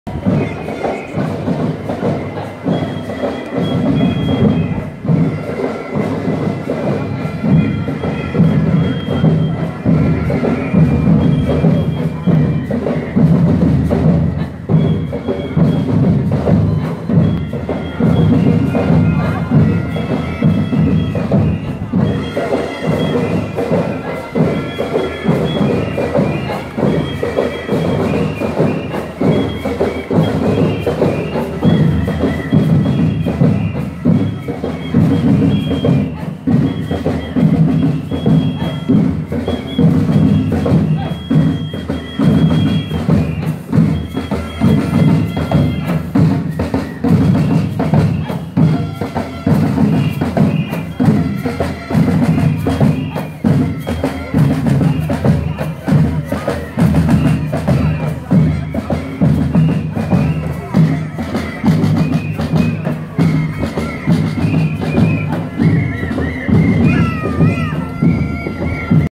1. Mai 2018 in Detmold- Demozug.
Der Demozug kommt an.